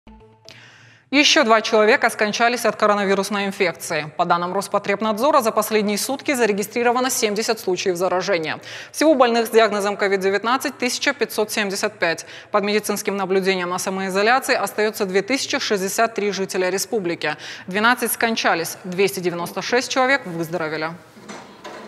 В Северной Осетии 1575 заболевших коронавирусом: 296 выздоровел и 12 умерли
Ещё два человека скончались от коронавирусной инфекции. По данным Роспотребнадзора, за последние сутки зарегистрировано 70 случаев заражения. Всего больных с диагнозом COVID-19 – 1575. Под медицинским наблюдением на самоизоляции остаётся 2063 жителя республики. 12 скончались, 296 человек выздоровели.
Мы рядом в:
ok.ru/irystontv